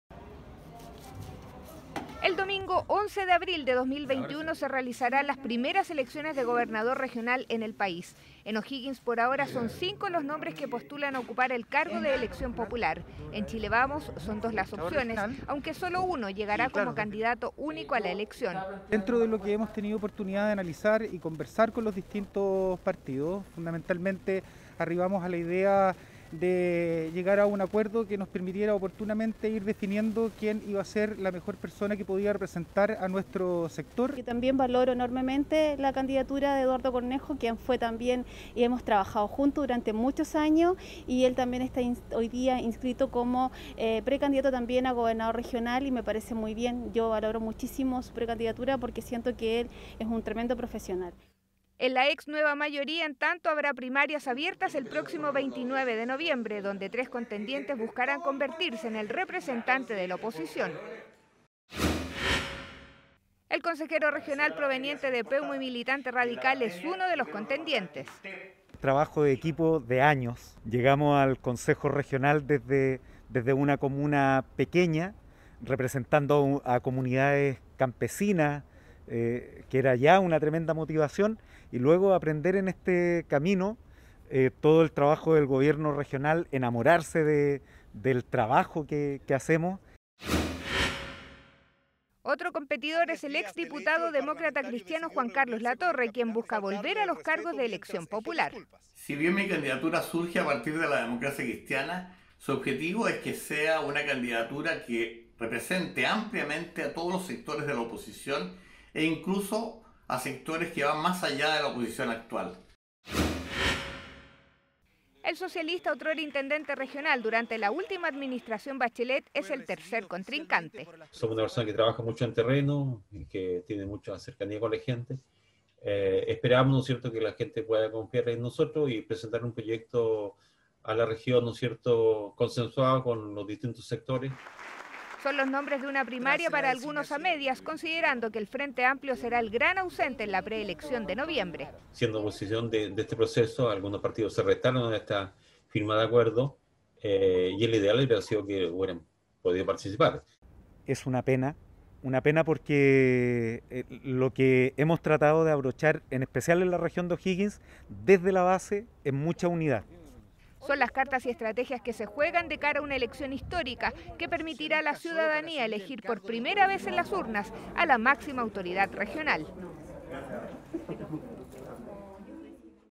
Comienza la carrera electoral por las gobernación regional
Partidos políticos ya tienen a sus candidatos quienes participarán en la elección primaria del próximo 29 de noviembre.